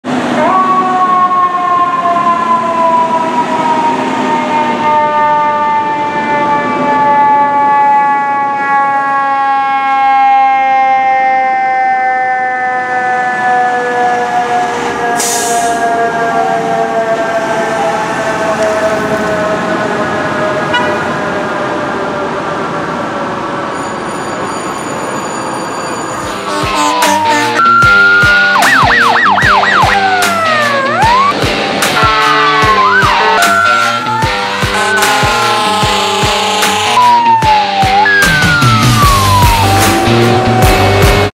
FDNY Engine 54 responding
Here you will see New York City Fire Department Engine 54 responding to an unknown call.

Midtown Manhattan
1/6/23